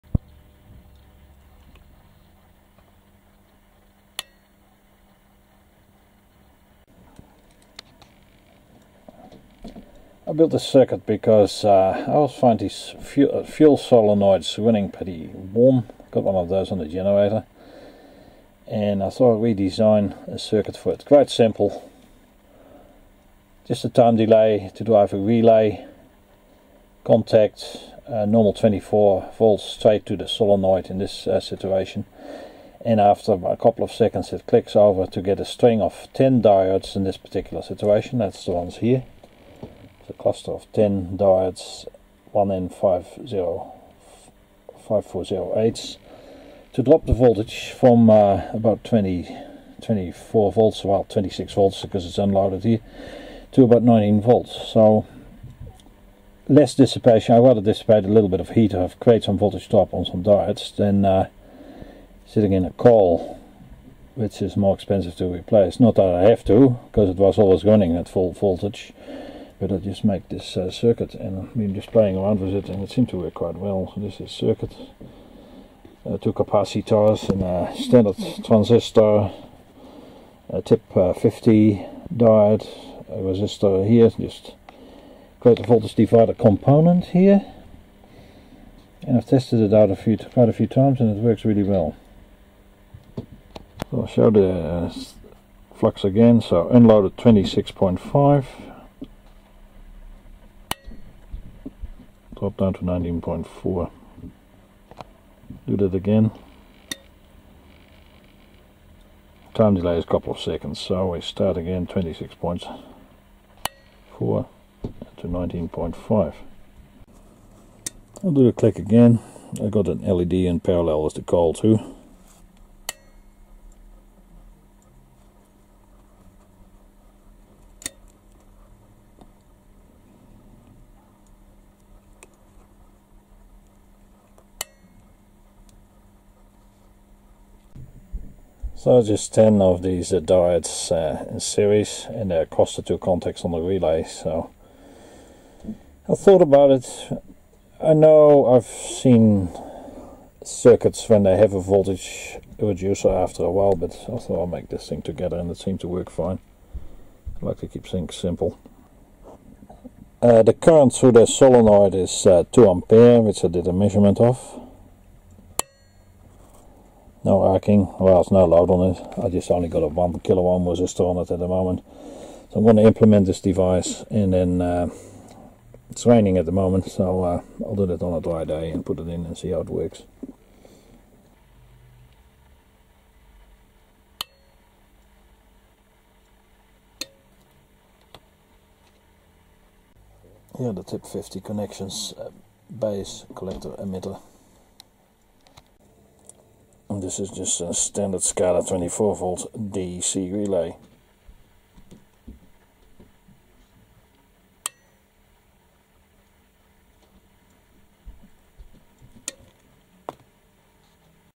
Voltage reducing timed relay for fuel solenoid.
Made this circuit up to run a fuel solenoid a bit cooler on a diesel engine.
It will reduce the initial voltage of 24 Volts by about 6 volts after the solenoid is energized.